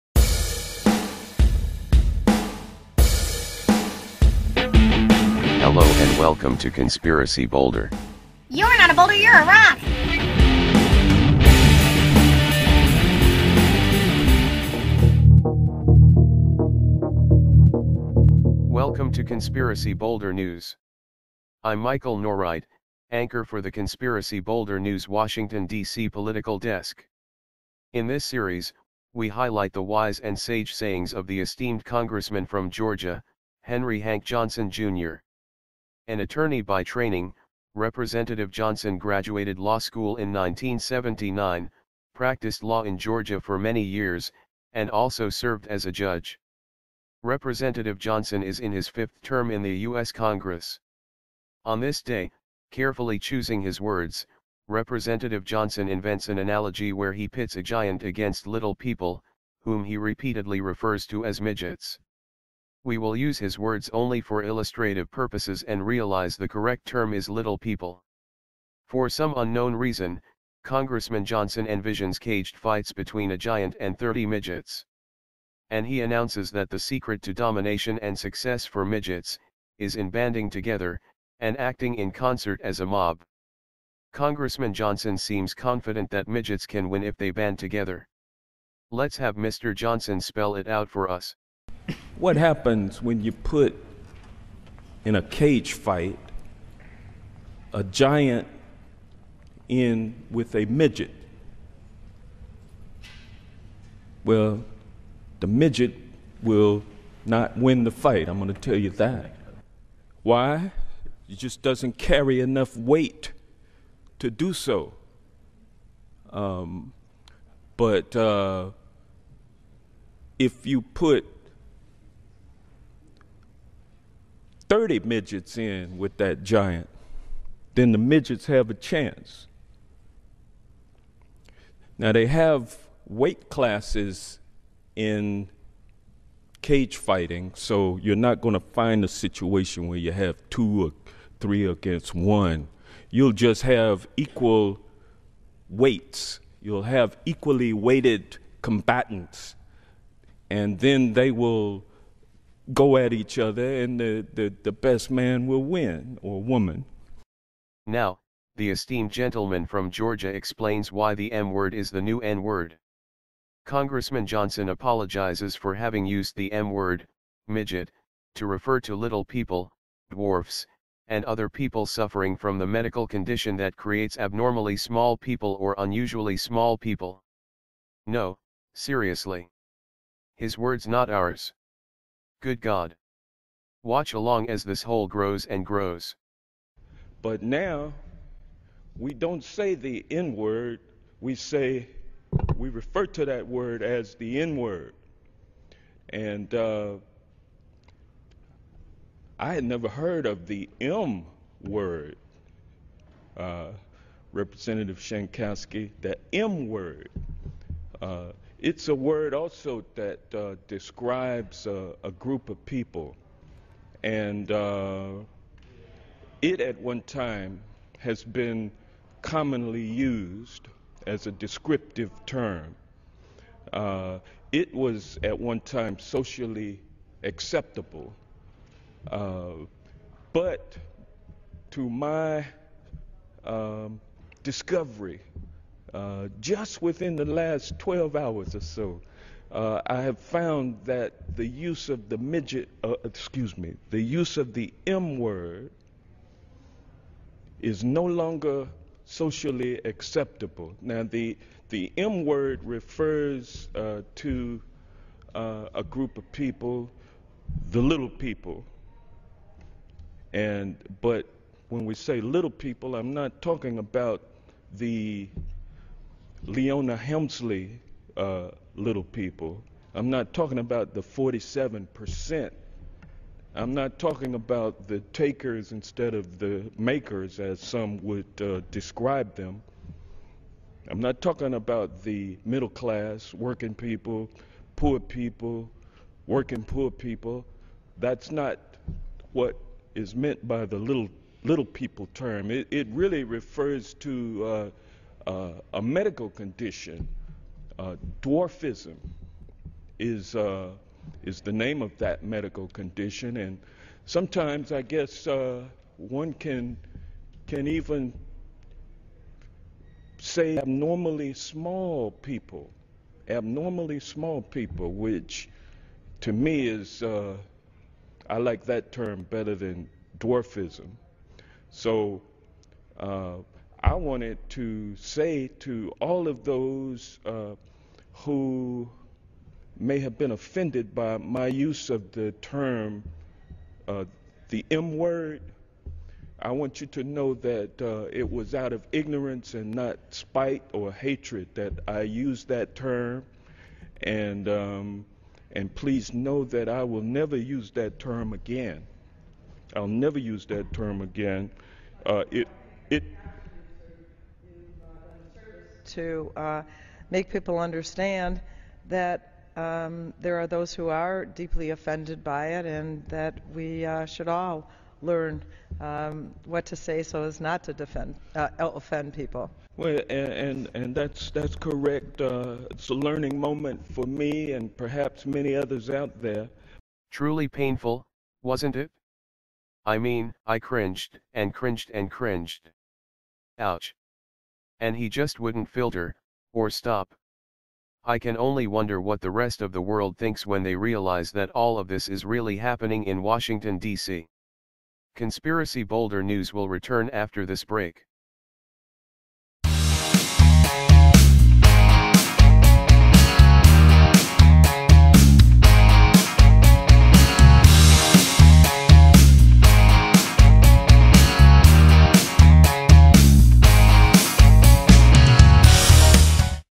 US Congressman Johnson on "Midgets vs Giants"
US Congressman Hank Johnson orates his newly created MIDGETS vs. GIANTS CAGE FIGHT analogy and later apologizes for using the "M" word. Along the way, the esteemed gentleman from Georgia repeats the offensive "M" word during his apology, and calls little people "abnormally small" and suffering from dwarfism. Painful, to say the least.

Another amazing scoop by Conspiracy Boulder Parodies! We're dumber than a bag of rocks!